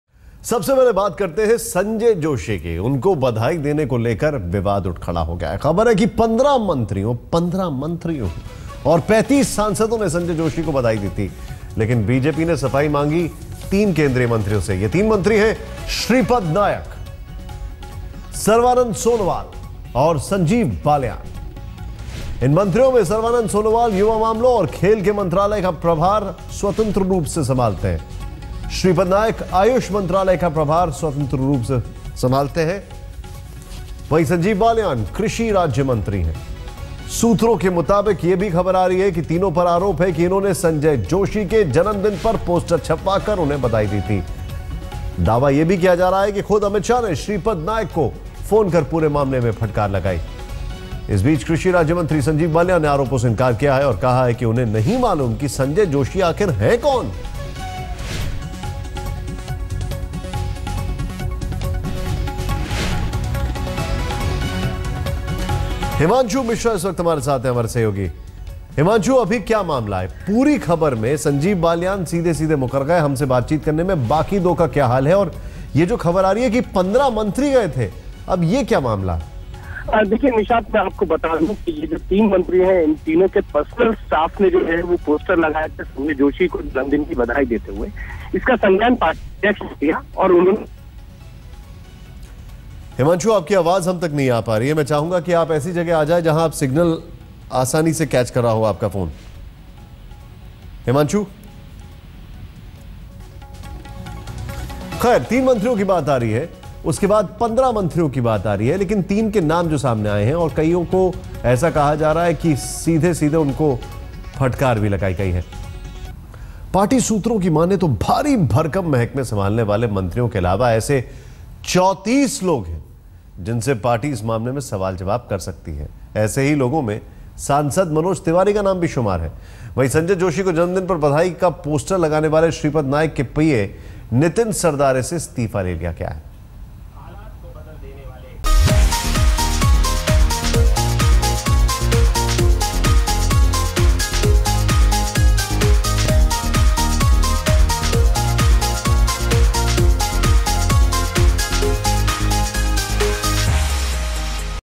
Sources- 15 BJP ministers Wished Sanjay Joshi On His Birthday
Minister's Shripad Naik, Sarvanand Sonewal & Sanjeev Balyan have been asked to justify their actions of wishing Sanjay Joshi on his birthday with posters.